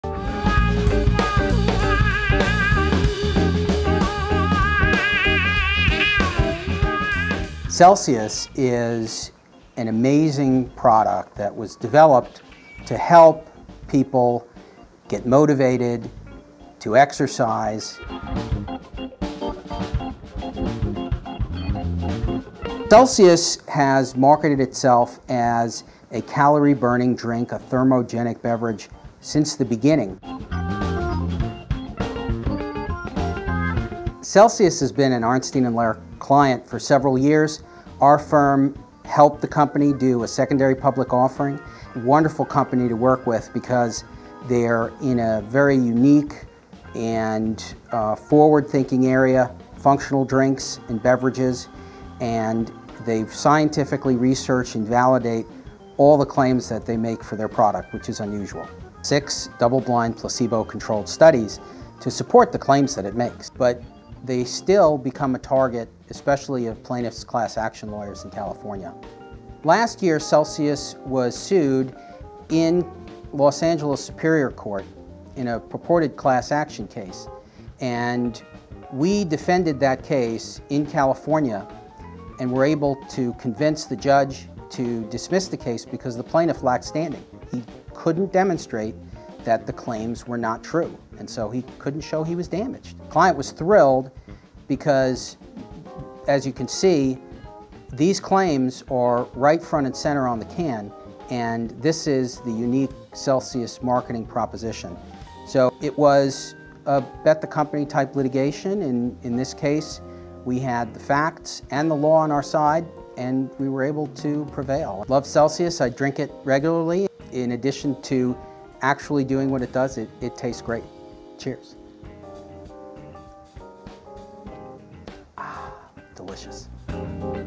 Joel Rothman -- Celsius Holdings
Joel Rothman discusses how Arnstein & Lehr represents Celsius